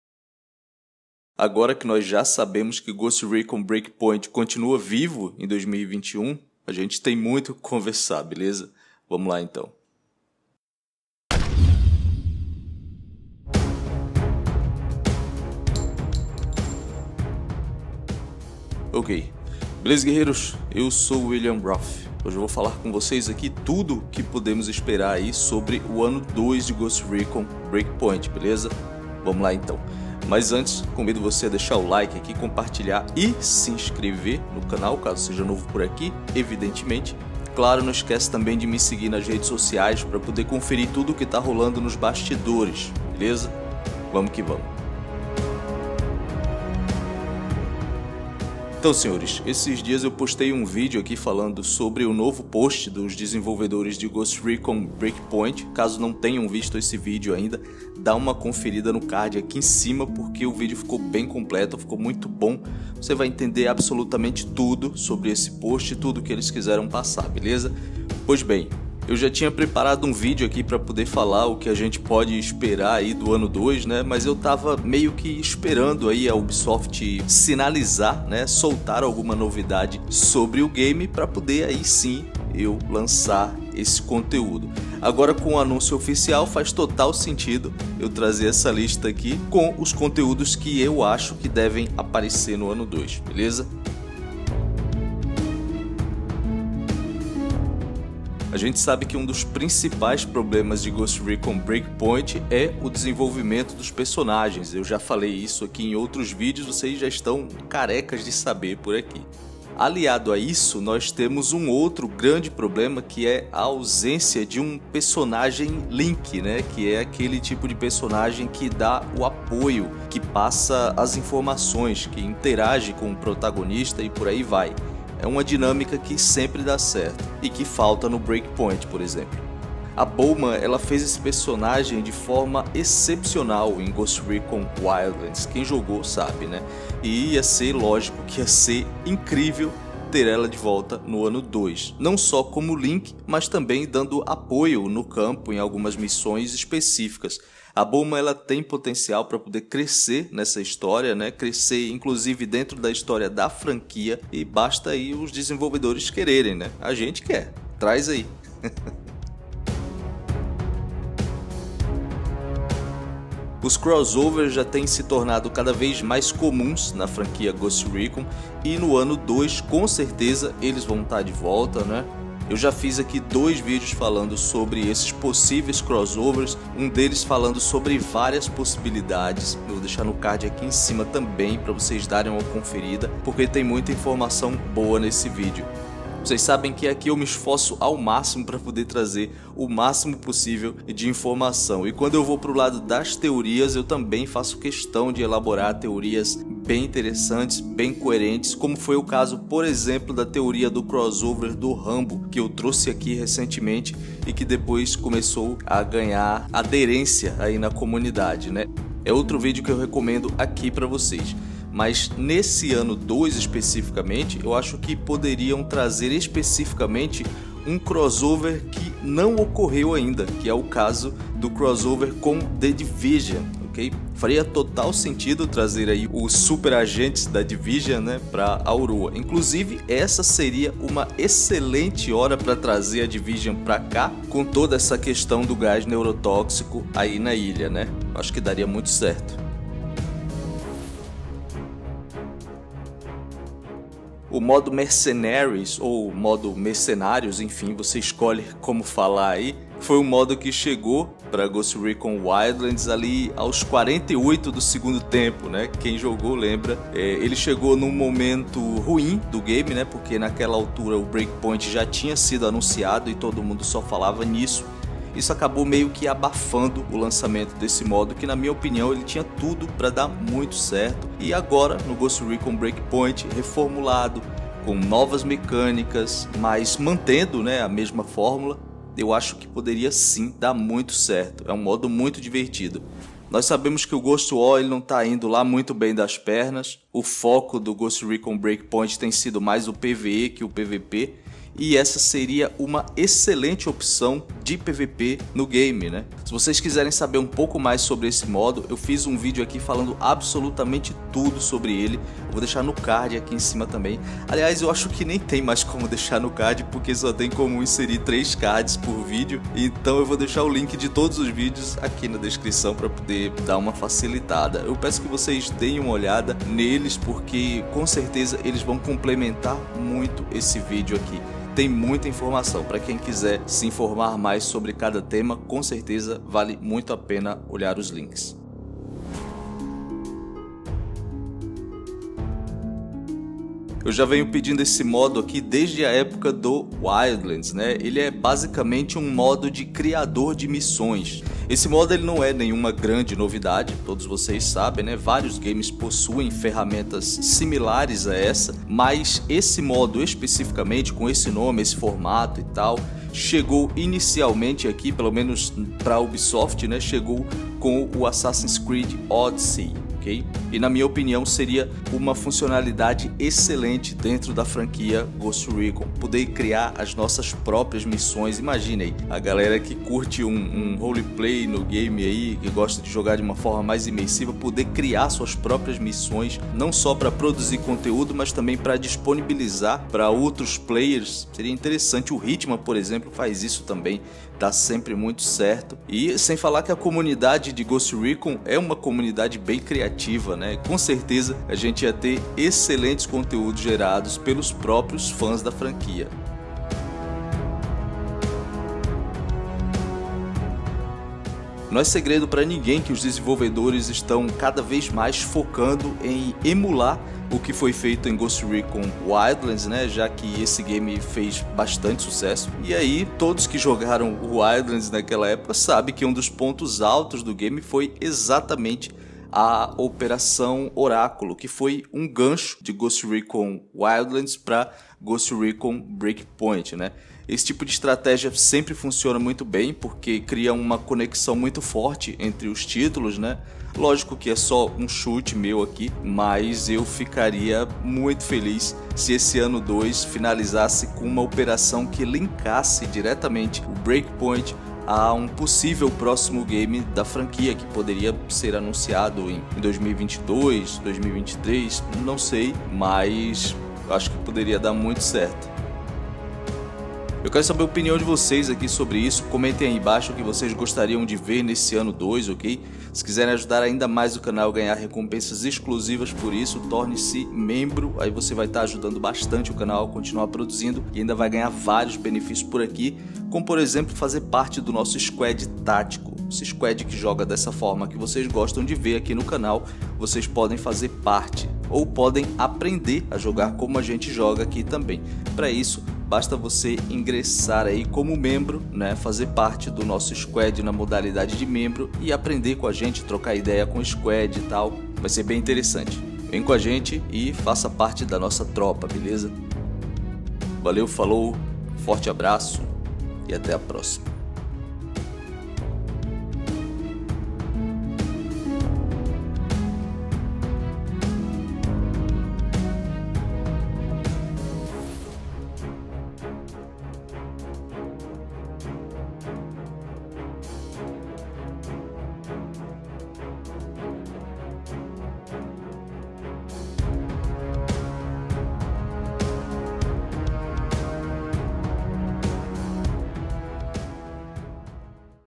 Ghost Recon Breakpoint ANO 2 – O que podemos esperar?
Eu já tinha preparado um vídeo pra falar sobre o que podemos esperar desse ano 2 de ghost recon breakpoint mas eu tava meio que esperando a ubisoft sinalizar soltar alguma novidade sobre o game pra soltar o vídeo. Agora com o anuncio oficial, do ano 2 de ghost recon breakpoint faz total sentido postar essa lista com os conteúdos que eu acho que devem aparecer no ano 2. Vamo lá.

PSN: willianrof